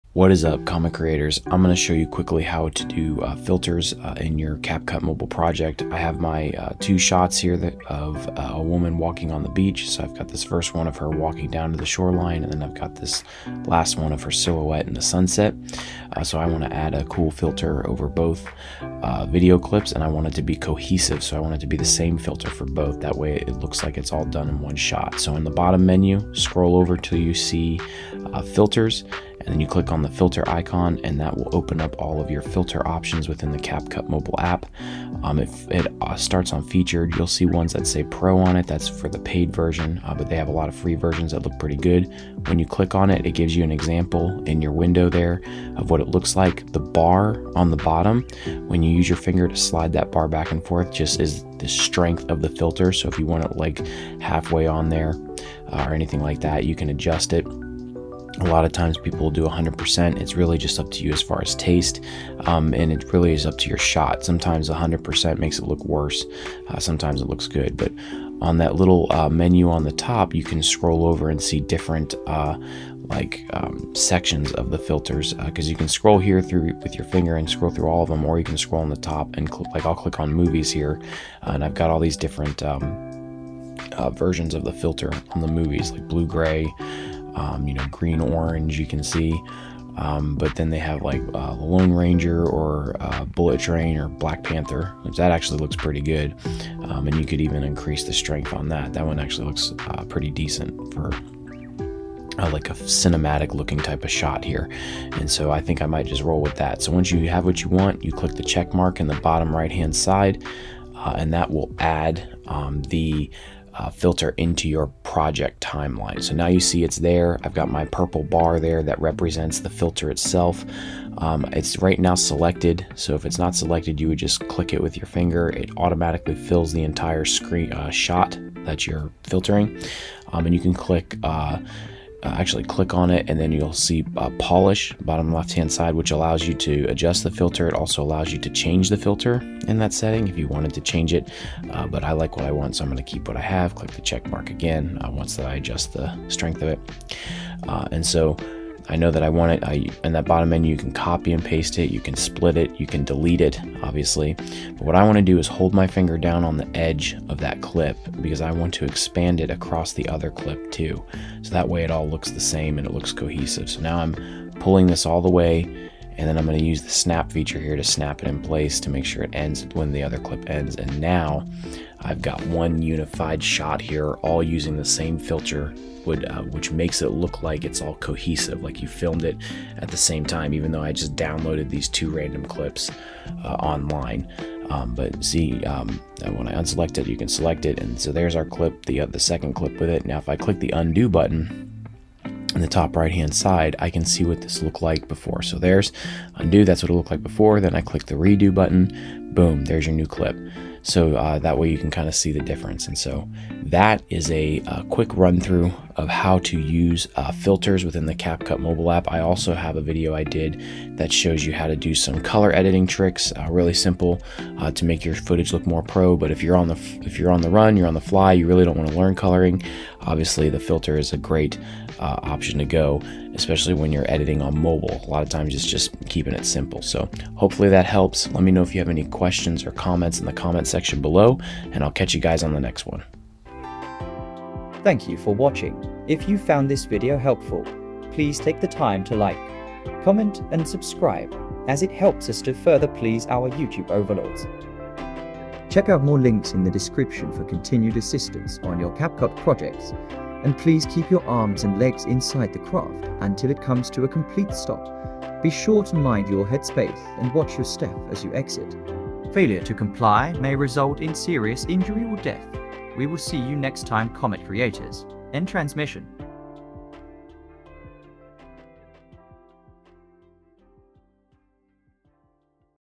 Use Filters In CapCut Made Simple (App) | IOS & Android
Does CapCut have filters? In this video we go over how to use filters in CapCut and how easy it is to use them on your entire project

Best filters for CapCut
lower strength of filters on CapCut